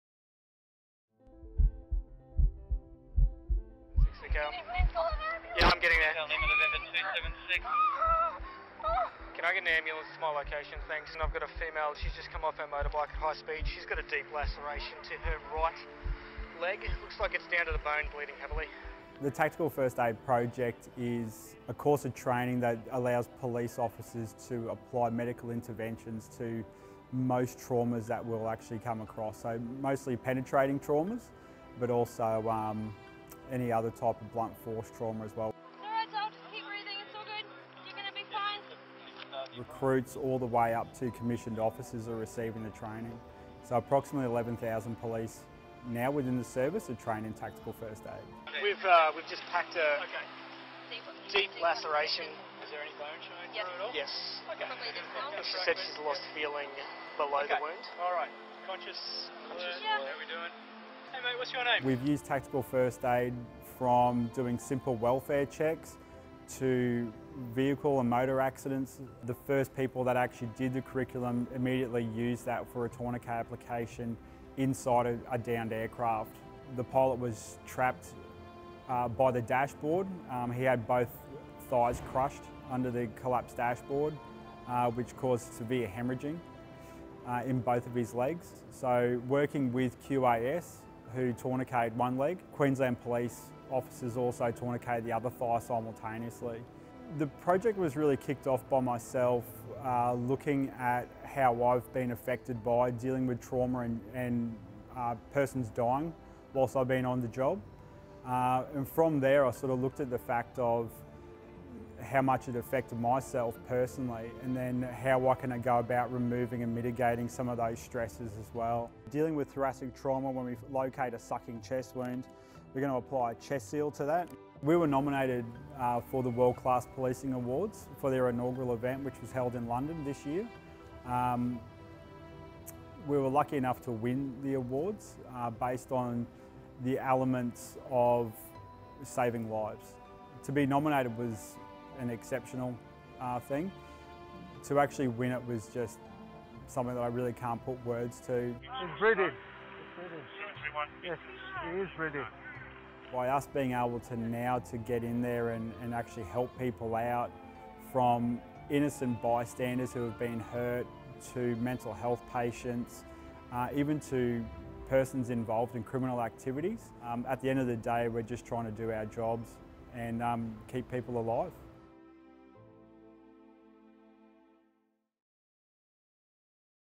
Tactical First Aid Project
A QPS project equipping officers with skills to deliver Tactical First Aid has been recognised on the international stage.
The project is providing huge benefits to the Queensland community with officers already putting their training into practise.